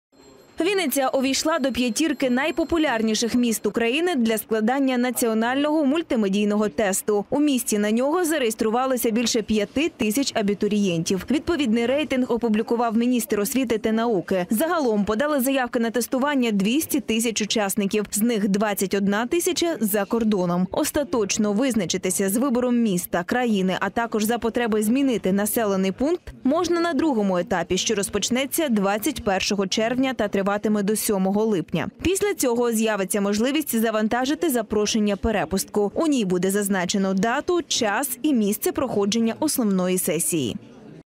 Вінниця увійшла до п’ятірки найпопулярніших міст для складання національного мультимедійного тесту
- Скільки у Вінниці зареєструвалося абітурієнтів?
- Коли завершиться реєстрація?
- Як відбуватиметься оцінювання якості освіти випускників?